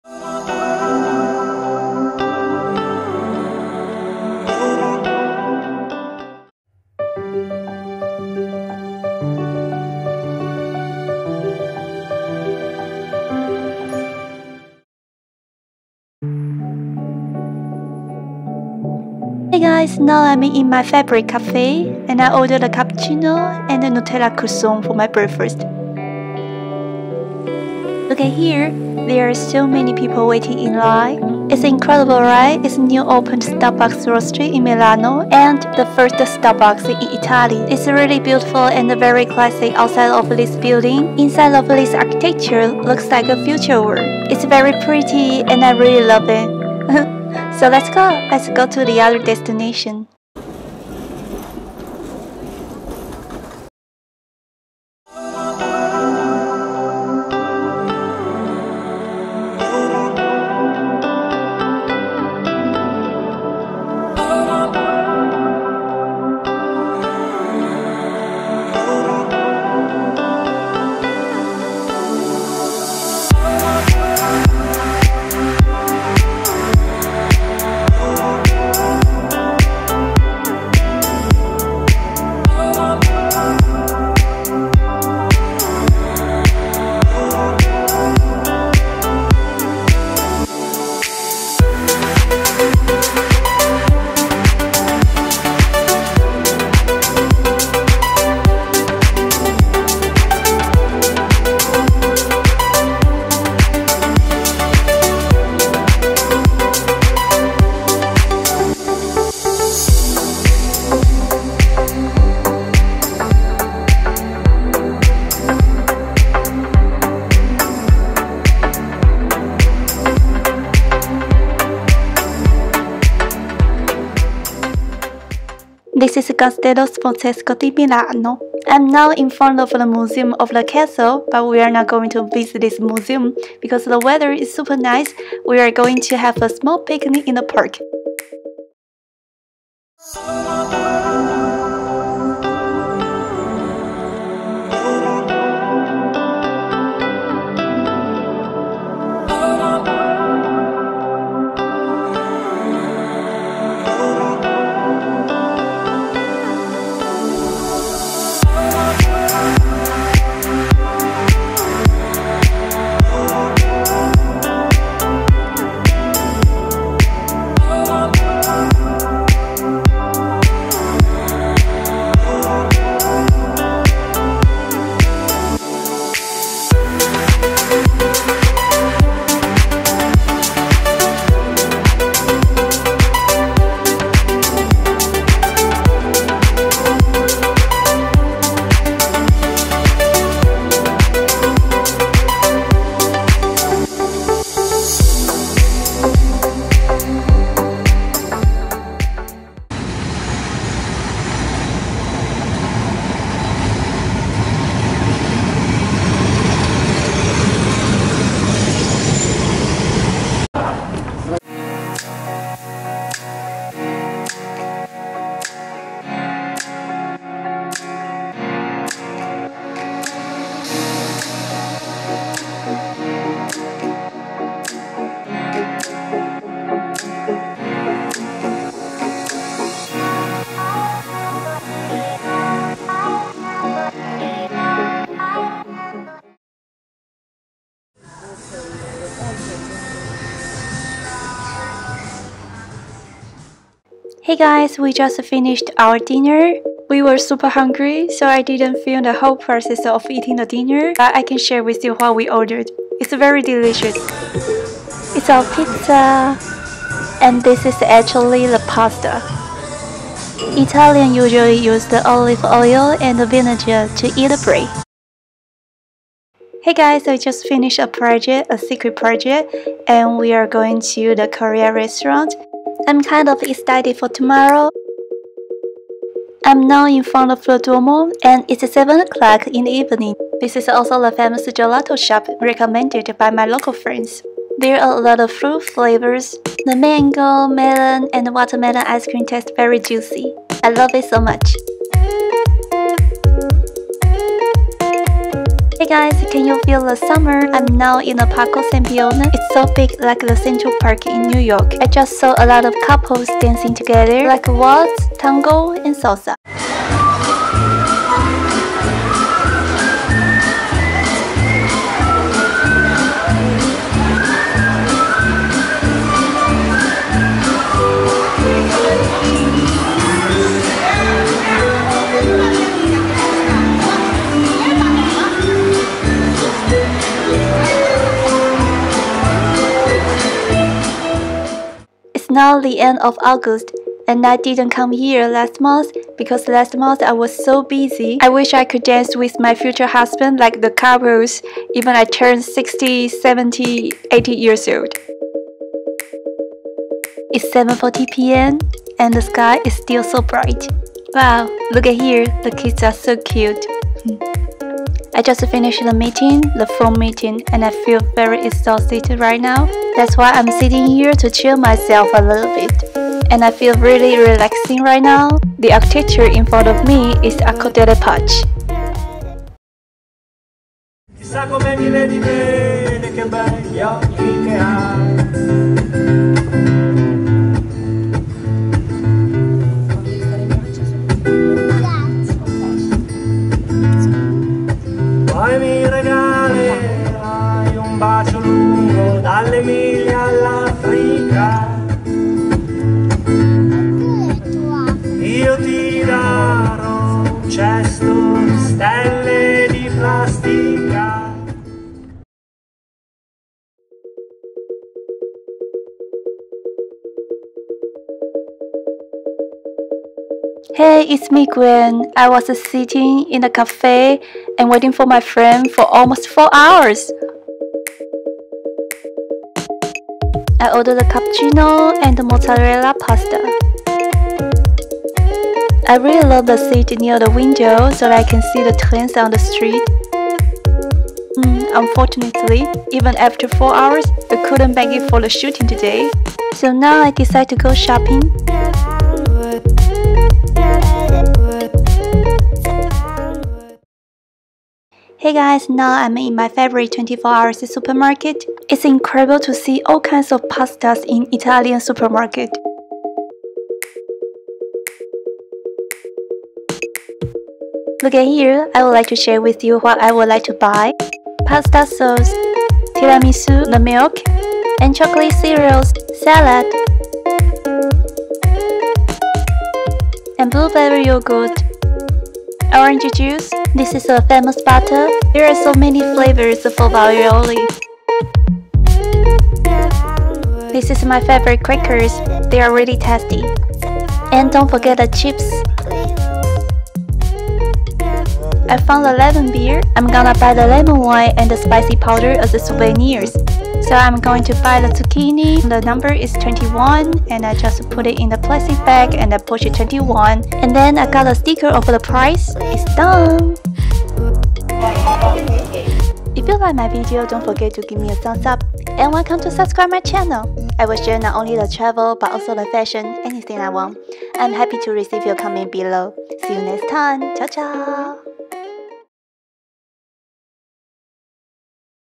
🇮🇹 MILAN Vlog 14: Summer Strolls at Castello Sforzesco, Parco Sempione & Arco della Pace!
I enjoyed a summer stroll from Duomo to Castello Sforzesco, starting at my favorite café, "Panini Durini." I’m obsessed with the vintage trams in Milan! After exploring Sforza Castle in the lovely weather, I chose to have a picnic in Parco Sempione. I walked down Via Dante, had dinner at Granaio Duomo, and treated myself to gelato at "Le Tre Gazzelle." The castle looked stunning at night, bathed in soft light, and I wrapped up my day at my favorite 24-hour supermarket!

→ Please turn on CC subtitles and select your language to start watching!